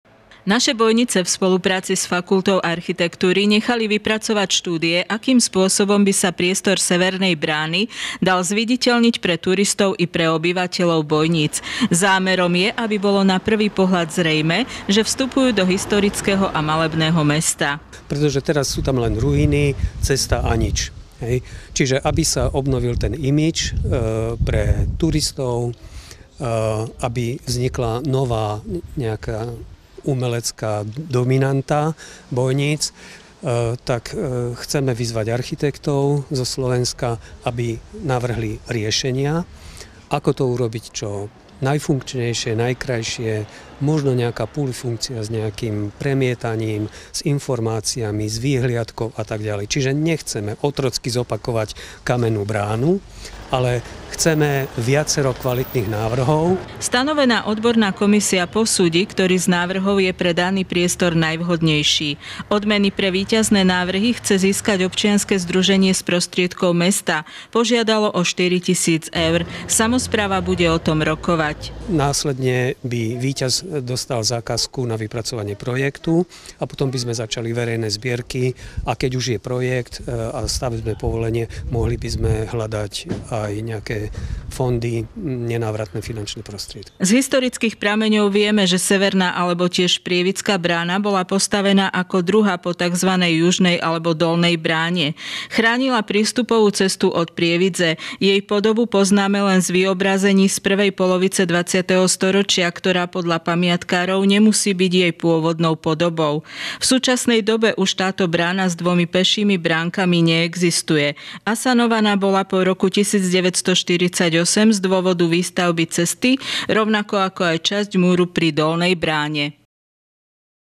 Vstup do Bojníc chcú zmeniť
Občianske združenie Naše Bojnice má vypracovaných viacero projektov na záchranu kultúrnych pamiatok v meste. Jedným z nich je i revitalizácia priestoru tzv. severnej brány. Cieľom je, aby vstup do kúpeľného mesta bol dôstojný.